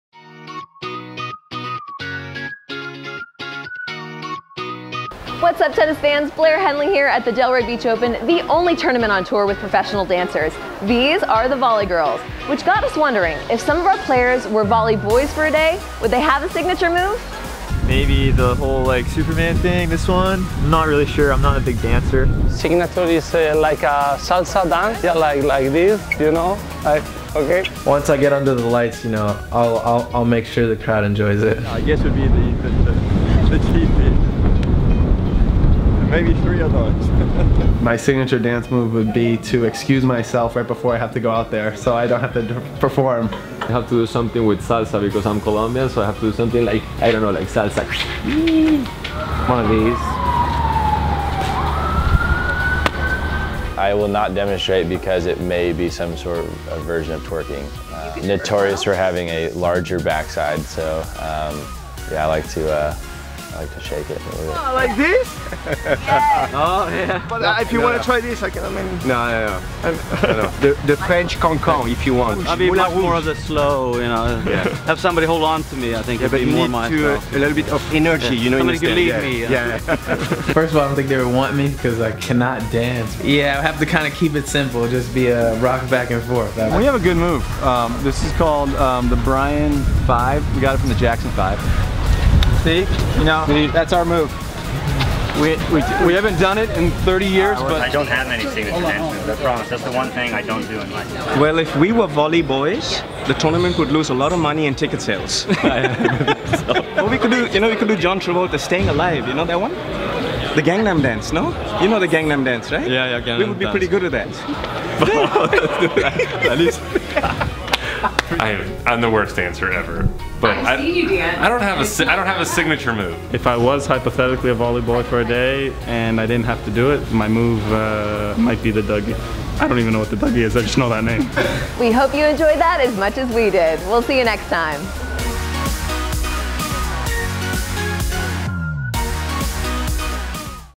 Dance Party: Top Players Show Us Their Moves
Watch top ATP tennis stars Milos Raonic, Jack Sock, the Bryan Brothers, Sam Querrey and more show off their signature dance moves.